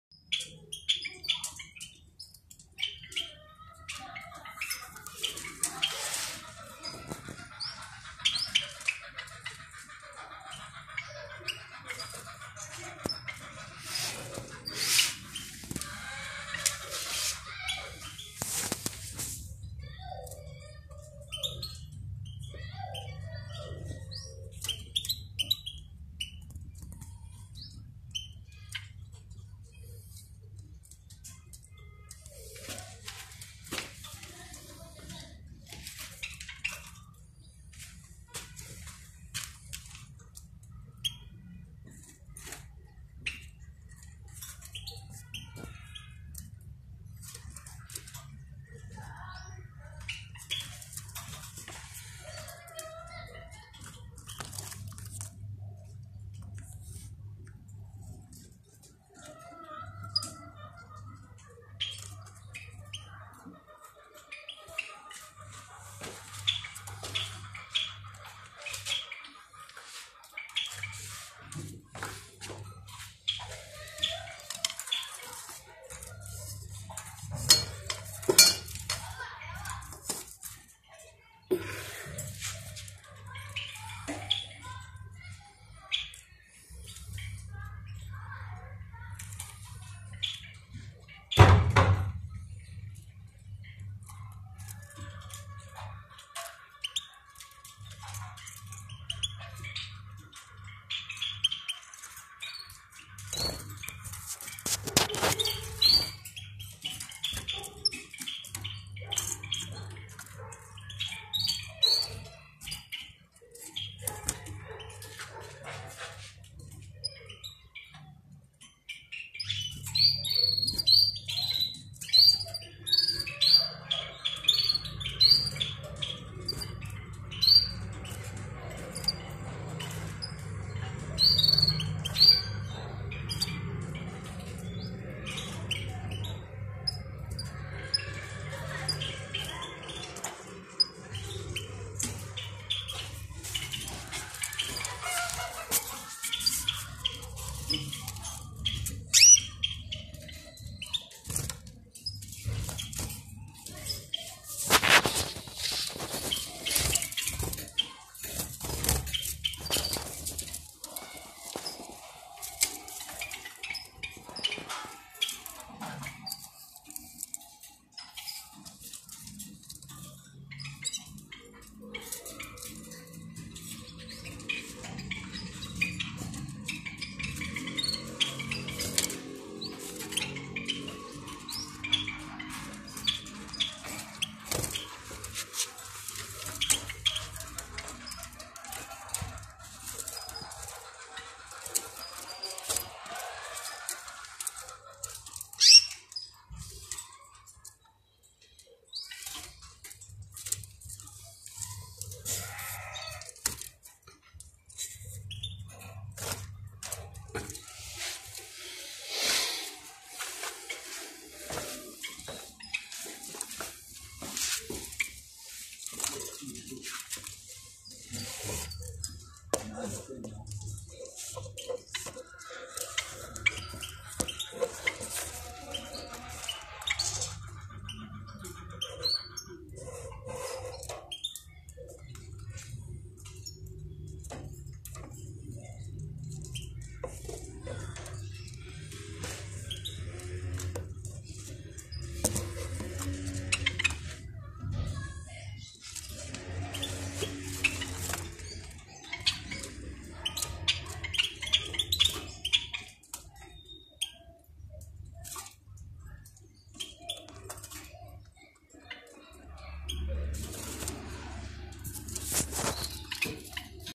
النموذج التطبيقي لعلوم طاقة الاشكال الهرمية في تربية طيور الزينة 1000/65
الباحث بروفيسور جمال السّوسي - تونس